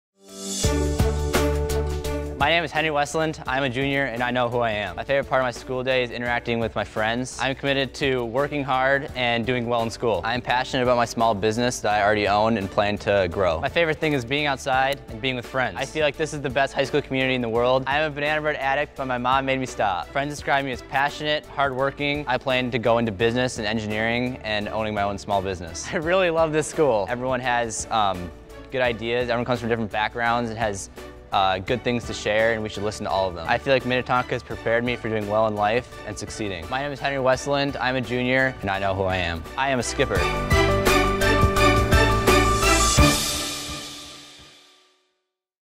I am a Skipper: Henry Westland
Minnetonka High School students share who they are, what they stand for and where they belong. The community at Minnetonka High School is unique; where students do the right thing and represent their school well.